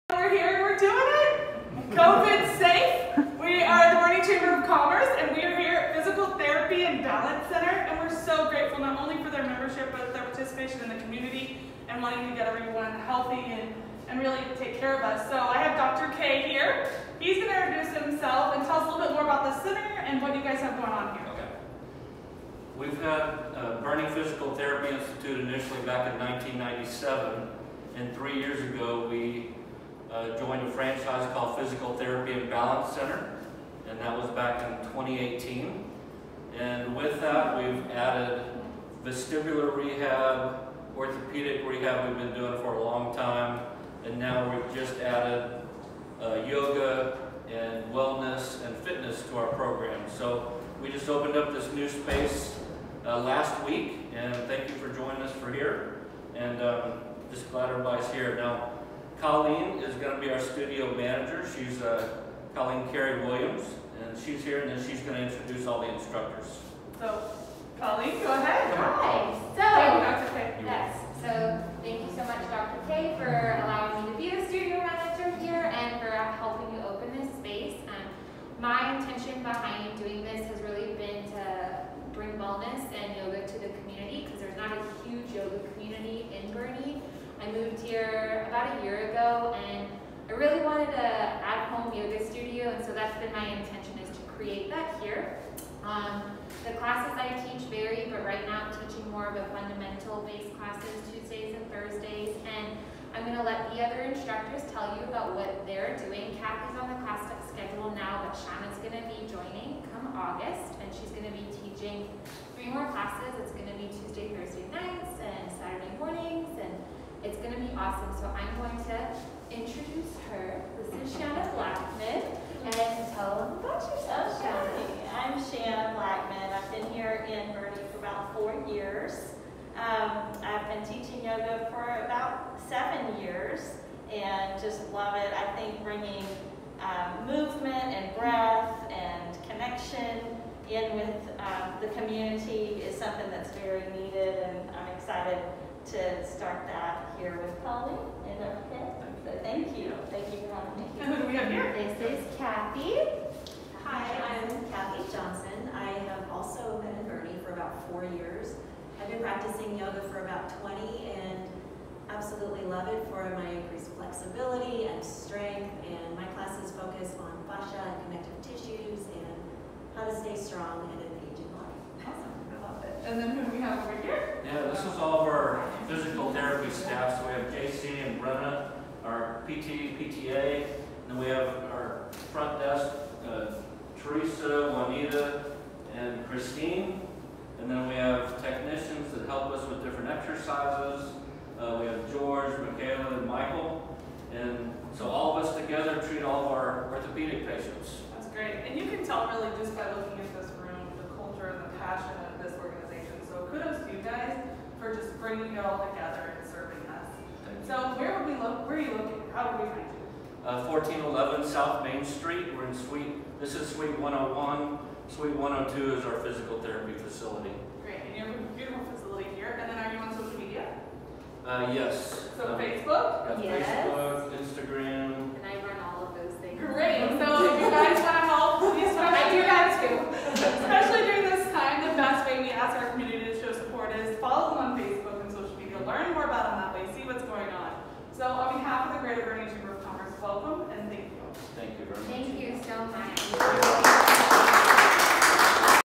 Ribbon Cutting for Fyzical Therapy & Balance Center
Were live with Fyzical Therapy & Balance Center to welcome them to our Chamber officially and celebrate them with a ribbon cutting!

Fyzical Therapy and Balance Center is a privately owned outpatient physical therapy clinic established in 1997. They offer hands on joint and soft tissue mobilizations, exercise and sports performance, as well as private fitness, yoga, and wellness classes.